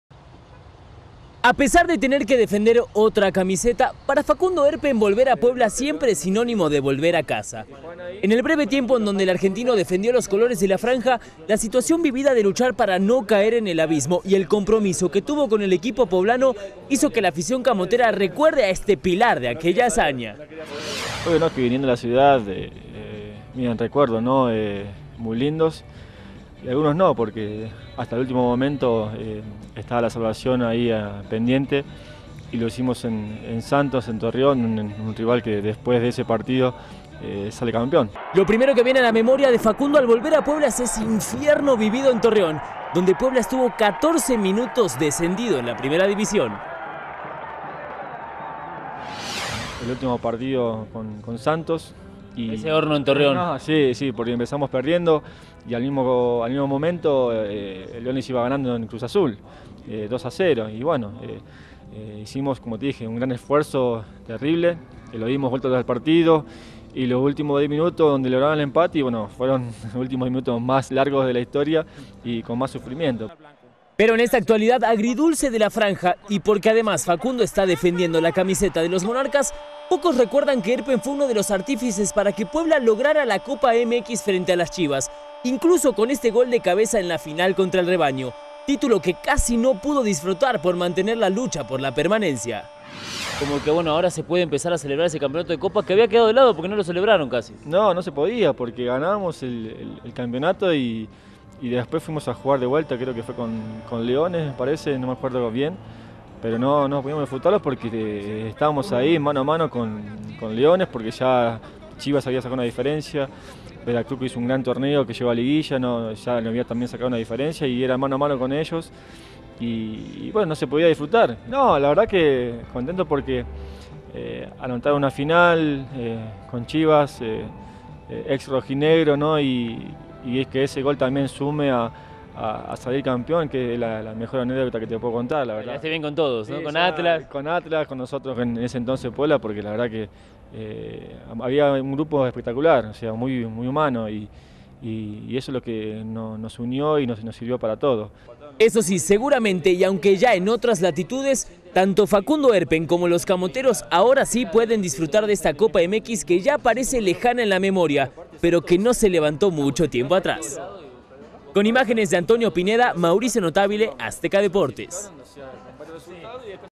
Semblanza Facundo Erpen
A pesar de que ya no defiende la camiseta del Puebla FC, Facundo Erpen siente a Puebla como su casa.